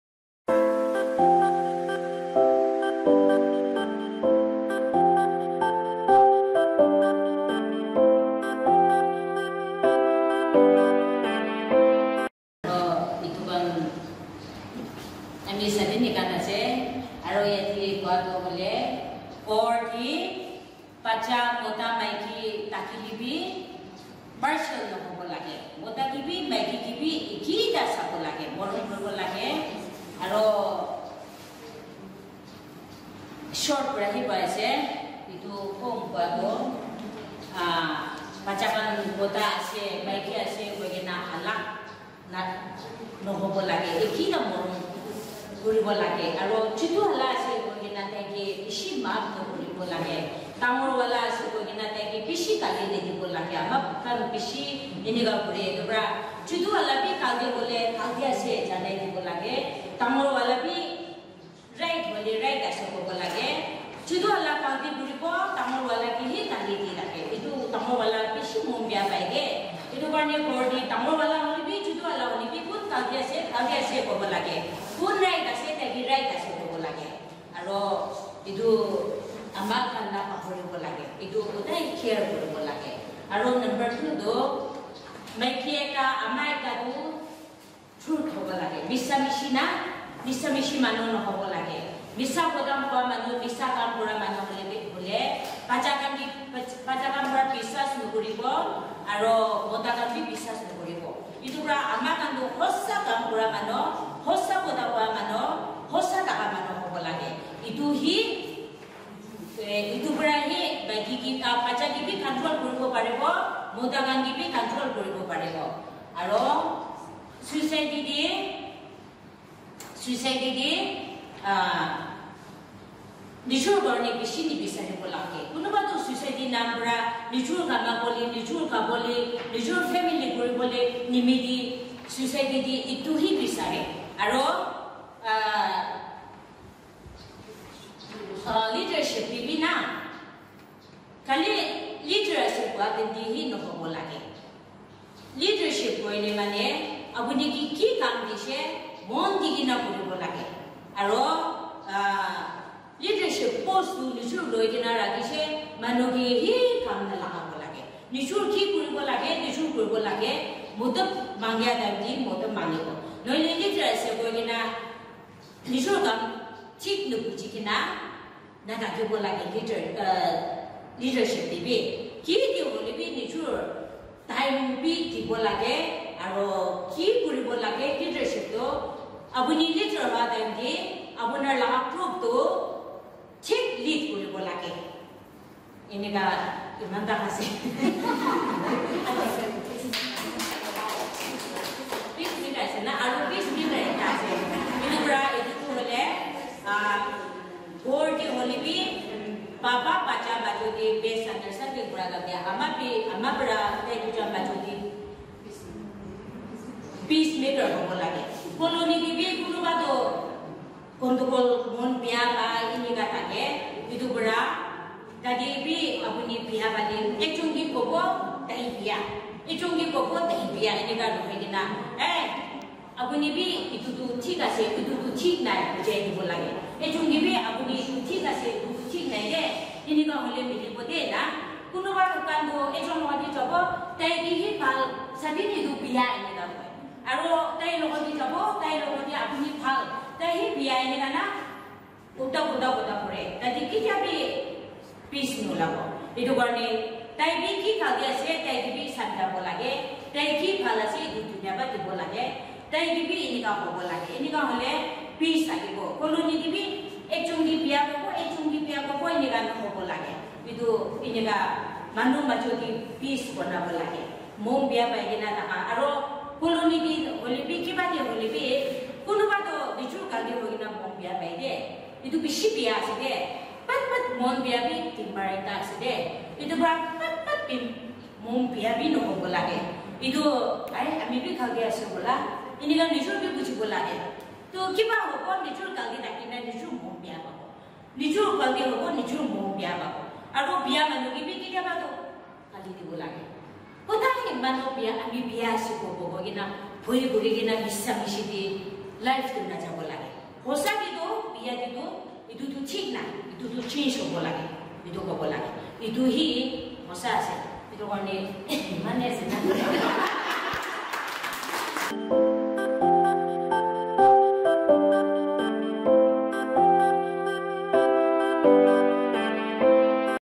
Peace Channel Nagaland: Role of Women in Peace Building at home and society at large
Nagamese Baptist Church Sieuruzha Colony Chumukedima Prsents Role of Woman in Peace Building at home and society at large